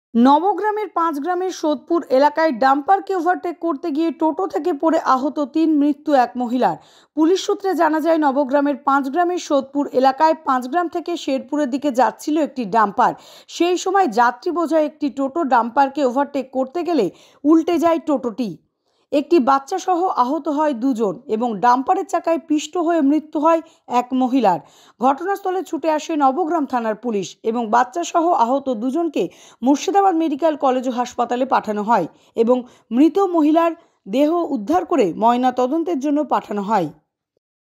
Nabagram Accident: ওভারটেক করতে গিয়ে টোটো থেকে পড়ে আহত তিন, মৃত্যু এক মহিলা নবগ্রামে
ঘটনাস্থলে ছুটে আসে নবগ্রাম থানার পুলিশ এবং ওই বাচ্চা সহ দুজনকে বহরমপুর মেডিকেল কলেজের পাঠানো হয় এবং এই মহিলার মৃতদেহকে ময়নাতনদের পাঠানোর জন্য নিয়ে আসে নবগ্রাম থানাই।